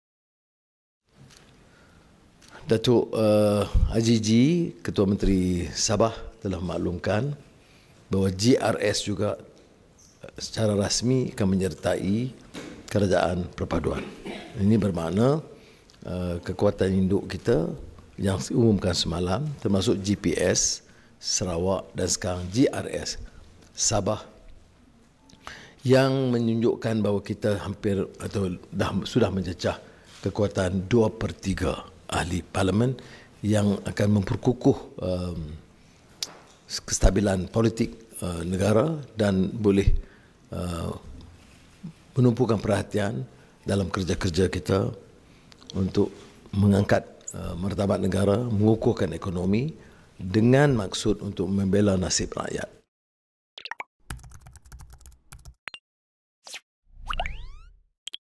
Kerajaan sudah capai sokongan dua pertiga, kata PM
Perdana Menteri Anwar Ibrahim berkata kerajaan perpaduan pimpinannya sudah mencapai sokongan dua pertiga susulan keputusan Gabungan Rakyat Sabah (GRS) bersetuju menyertai pentadbirannya.